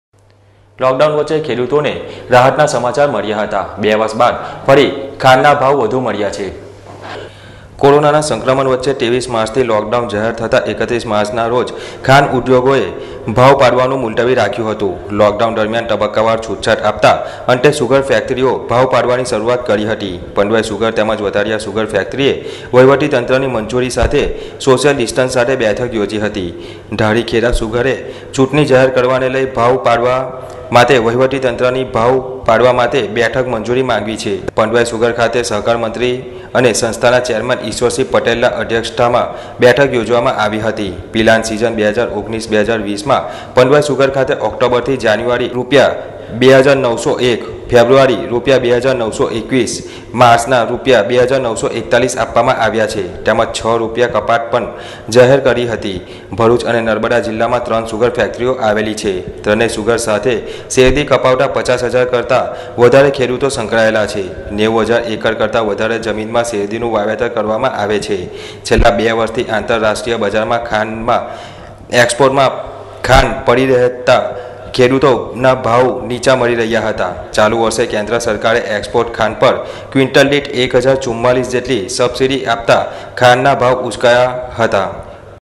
માં ટી.વી. 01-06-2020
લોક ડાઉન વચ્ચે ખેડૂતો મળ્યા રાહત ના સમાચાર : 2 વર્ષ બાદ ફરી ખાંડના ભાવ વધુ મળ્યા